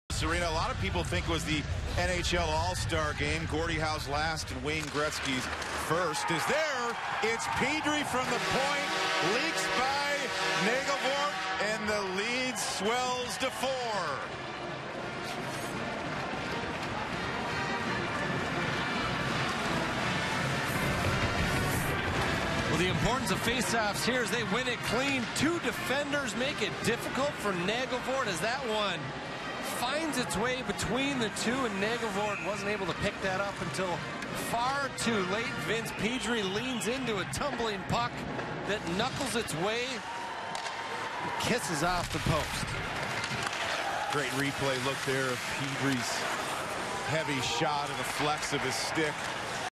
Vince Pedrie Kisses the Goal Off the Post vs. Michigan
Penn State's Vince Pedrie scores the Nittany Lions fourth goal of the evening vs. Michigan.